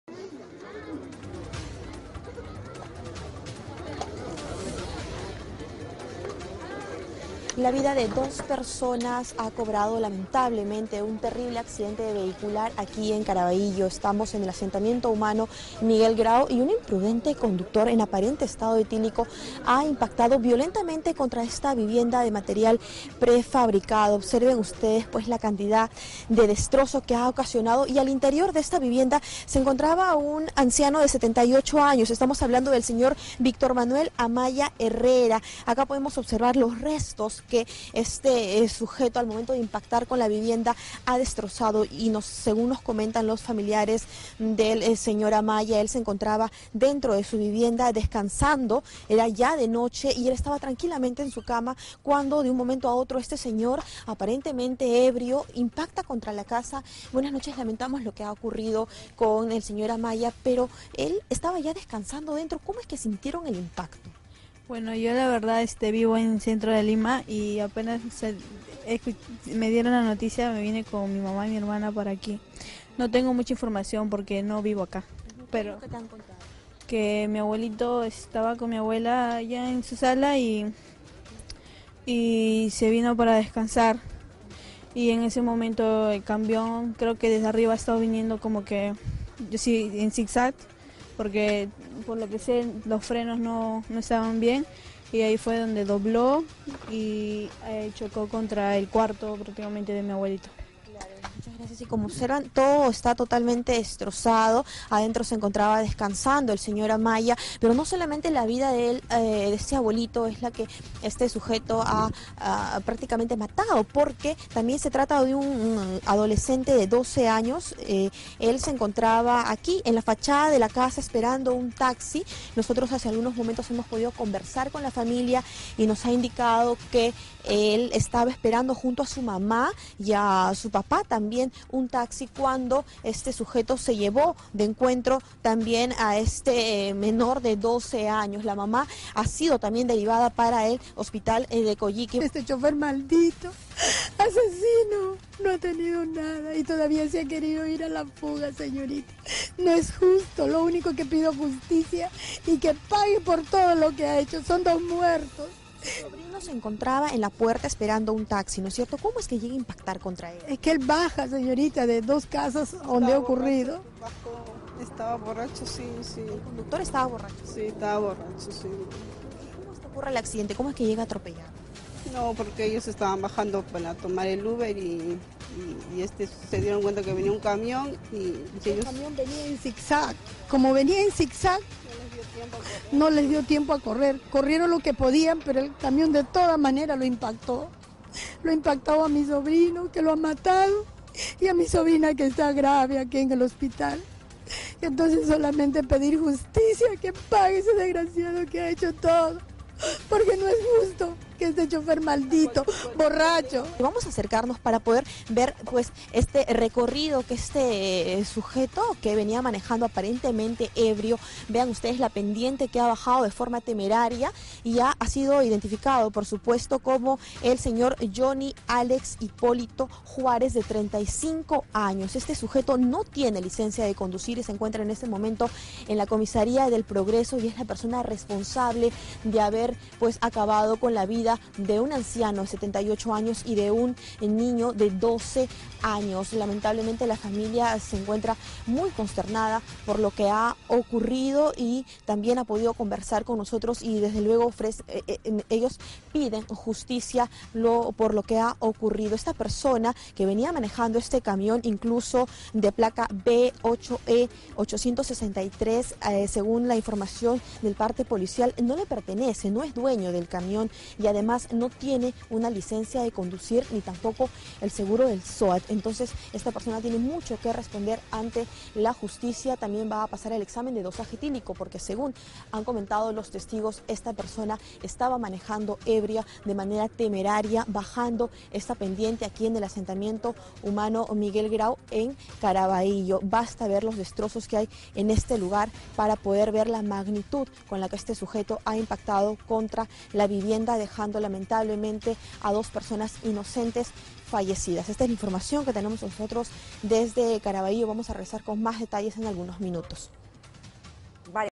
Carabayllo: sujeto aparentemente ebrio choca camión y mata a dos personas (1/2)
Esta madrugada, un conductor en aparente estado de ebriedad mató a un niño de 12 años que esperaba un taxi junto a sus padres en el distrito de Carabayllo. Este camión se empotró contra una vivienda donde provocó la muerte de un anciano.

El conductor de la unidad fue identificado como Jhony Álex Hipólito Juárez de 35 años, quien descendía por una pendiente cuando perdió el control del camión y arrollo a un niño de doce años. La madre del menor también resultó herida.

Luego impactó la unidad contra una vivienda de material prefabricado del asentamiento humano “Miguel Grau” y acabó con la vida de Víctor Manuel Amaya Herrera de 78 años, quien descansaba en el interior de la casa.

El adulto mayor falleció al instante mientras que la madre del niño quedó herida de gravedad y fue trasladada hasta el hospital de Collique. Tras el accidente, Hipólito Juárez intentó darse a la fuga y evadir su responsabilidad.

Cabe resaltar que el conductor no posee licencia para conducir y se encontraba en presunto estado de ebriedad. Las familias exigen justicia y que caiga todo el peso de la ley contra este irresponsable sujeto.

2